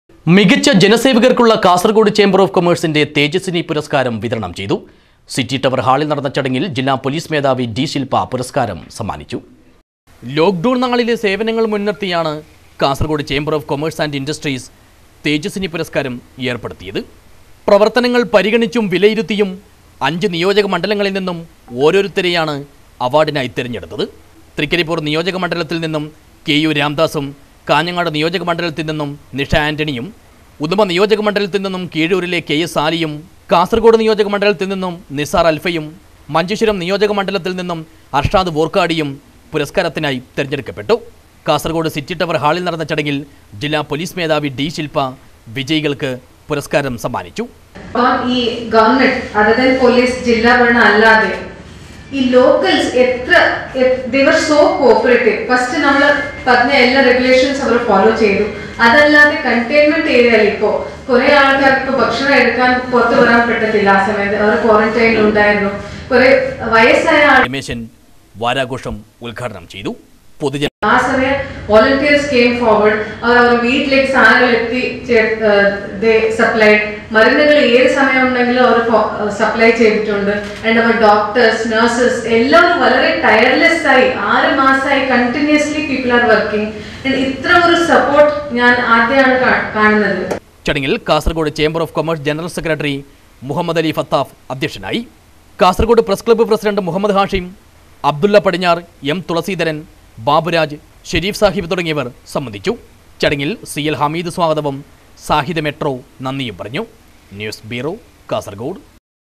മികച്ച ജനസേവകര്‍ക്കുള്ള കാസര്‍ഗോഡ് ചേമ്പര്‍ ഓഫ് കൊമേഴ്‌സിന്റെ തേജസ്വിനി പുരസ്‌കാരം വിതരണം ചെയ്തു.
മികച്ച ജനസേവകര്‍ക്കുള്ള കാസര്‍ഗോഡ് ചേമ്പര്‍ ഓഫ് കൊമേഴ്‌സിന്റെ തേജസ്വിനി പുരസ്‌കാരം വിതരണം ചെയ്തു. സിറ്റി ടവര്‍ ഹാളില്‍ നടന്ന ചടങ്ങില്‍ ജില്ലാ പൊലീസ് മേധാവി ഡി.ശില്പ പുരസ്‌കാരം സമ്മാനിച്ചു.